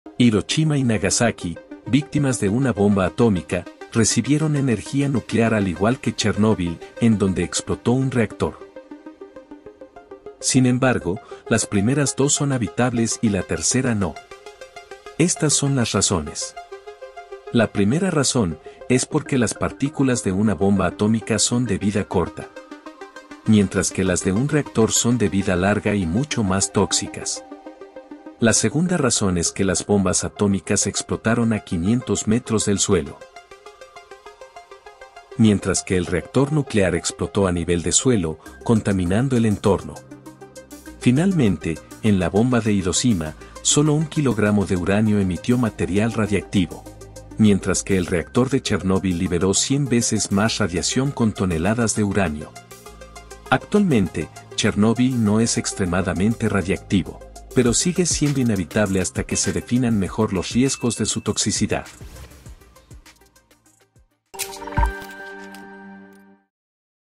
¿Por qué Hiroshima y Nagasaki son seguros pero Chernóbil no?
Aunque Chernóbil estuvo en contacto con energía nuclear como Hiroshima y Nagasaki, hay tres razones por las que éstas son habitables.

00:00 Las tres razones por las que Hiroshima y Nagasaki son habitables pero Chernóbil no
00:18 Razón 1: una bomba y un reactor funcionan distinto
00:30 Razón 2: la altura desde la que se lanzaron las bombas
00:42 Razón 3: en Chernóbil cayó más material radiactivo
00:53 Chernóbil no es tan tóxico, pero sigue siendo inhabitable